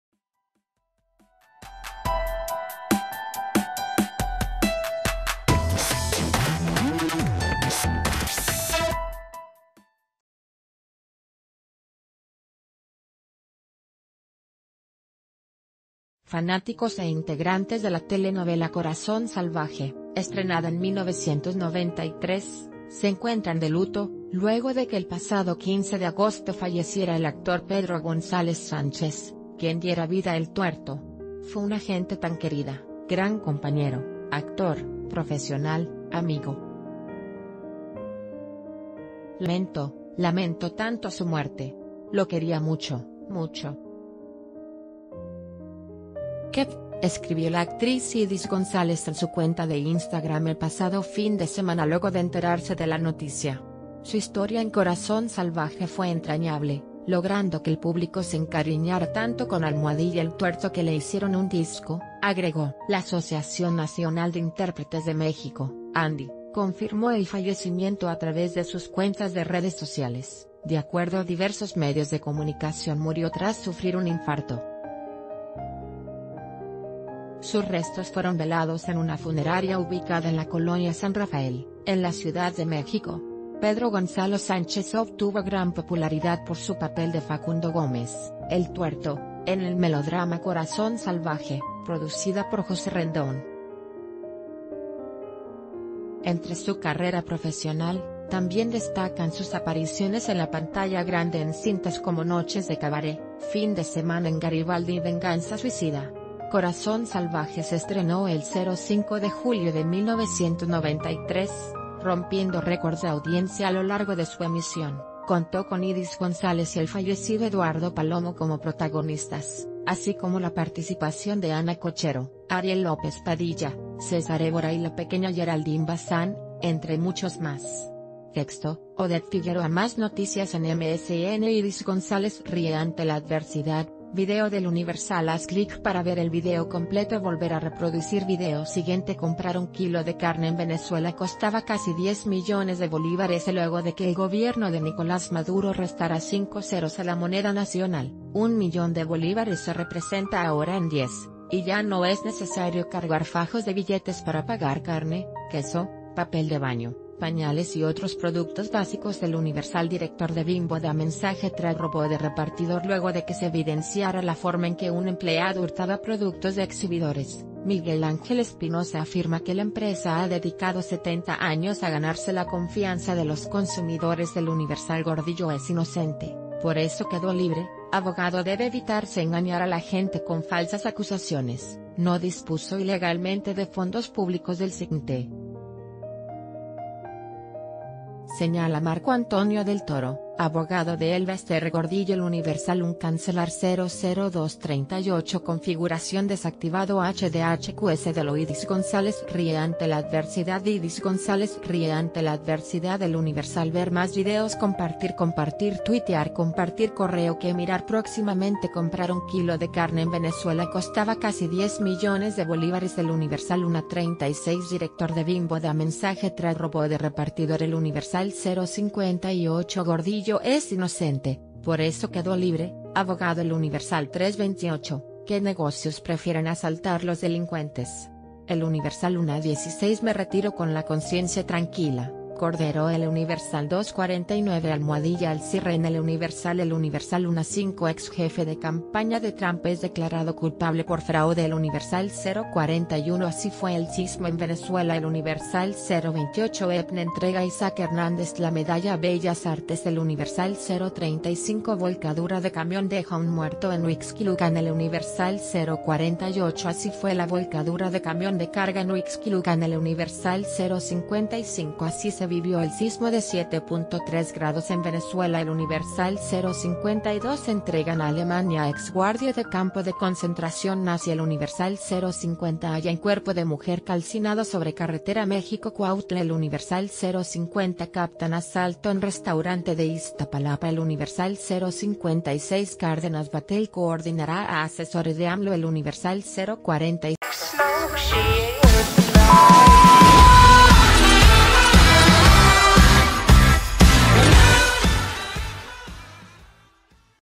Fallece actor de Corazón Salvaje
Fanáticos e integrantes de la telenovela Corazón Salvaje, estrenada en 1993, se encuentran de luto, luego de que el pasado 15 de agosto falleciera el actor Pedro Gonzalez Sánchez, quien diera vida a El Tuerto.“Fue una gente tan querida, gran compañero, actor, profesional, amigo. Lamento, lamento tan...